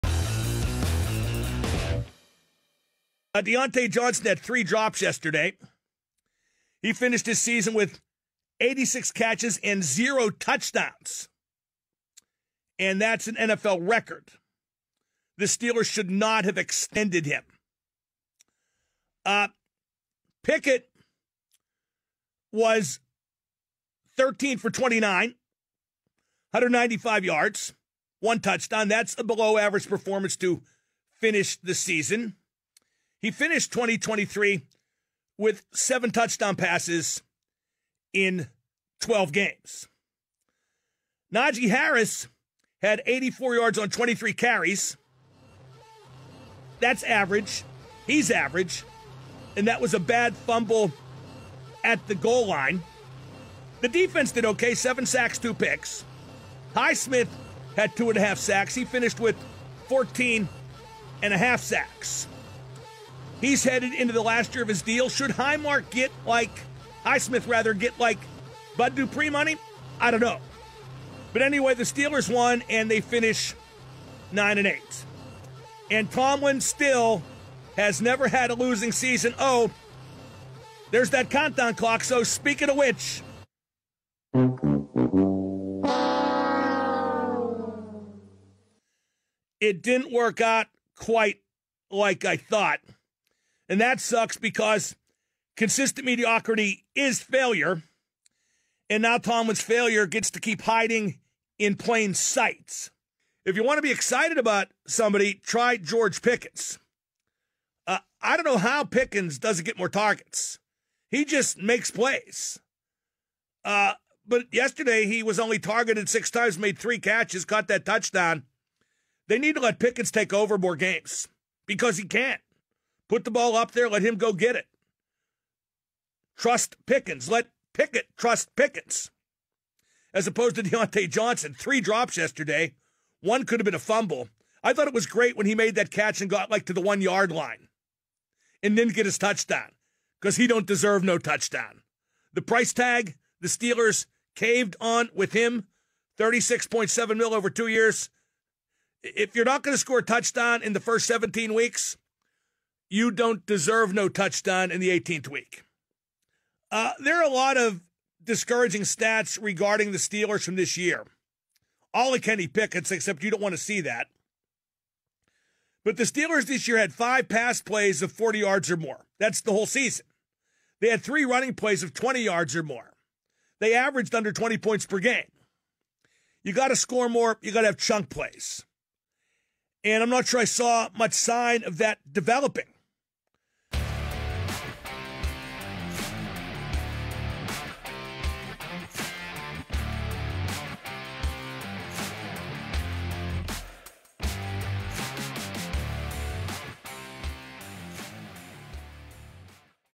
Steelers Season Recap - Mark Madden
Straight up, Mark Madden gives you his take on the Steelers' season.